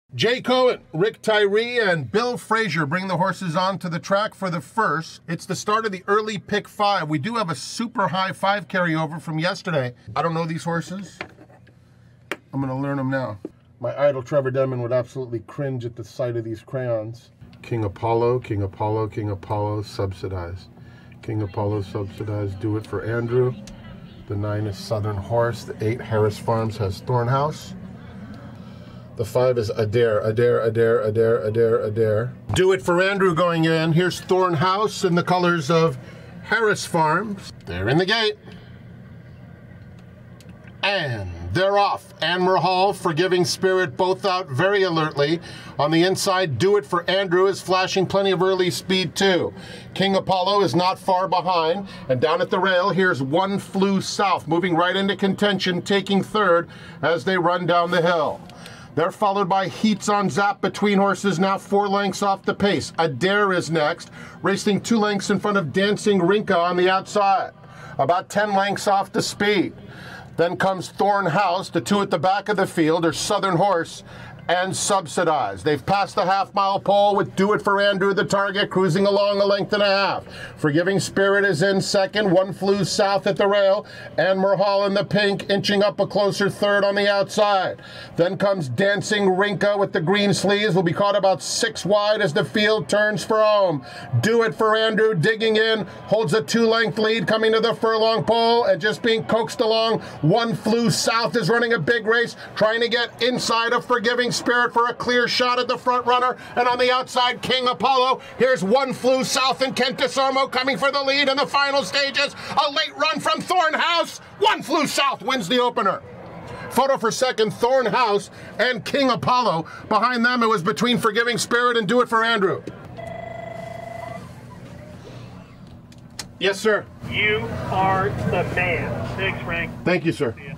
Behind The Scenes with Frank Mirahmadi - Live Santa Anita Race Call
Santa Anita Park track announcer let America's Best Racing into the announcer's booth as he called a race on RUNHAPPY Santa Anita Derby Day. Watch as he shares his process for remembering the horses running in each race and how he reacts to the unfolding race.